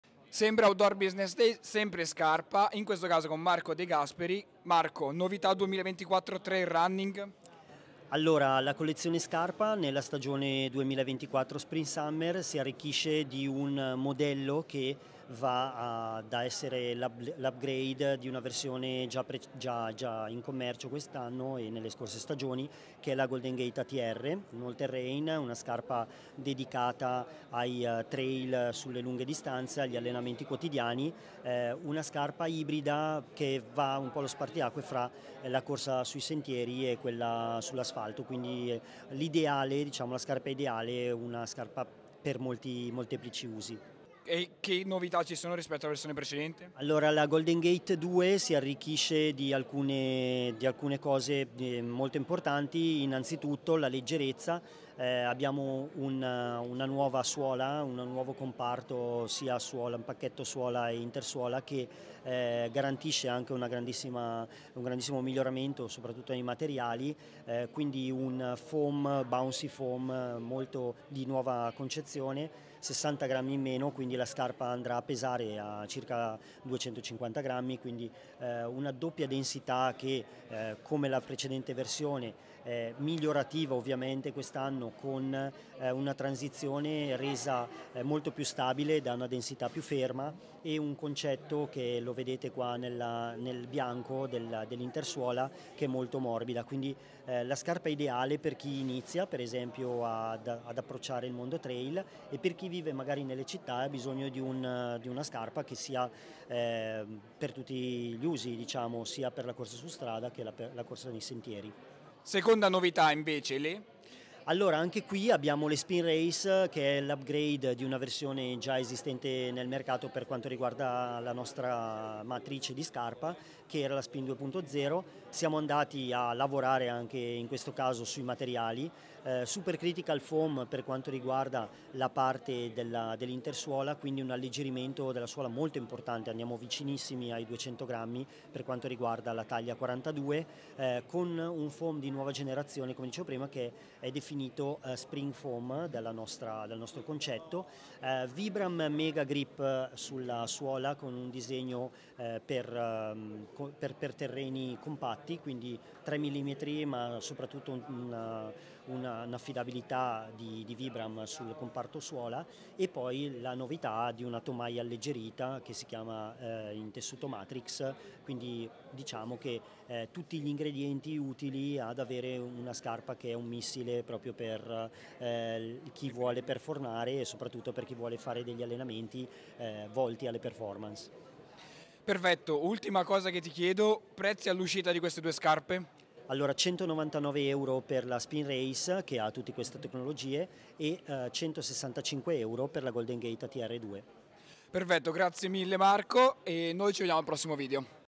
Novità Scarpa Trail Running 2024 | Scarpa Spin Race e Scarpa Golden Gate ATR 2
Oggi vediamo le novità 2024 da trail running di Scarpa, nel particolare i modelli Scarpa Spin Race e Scarpa Golden Gate ATR 2.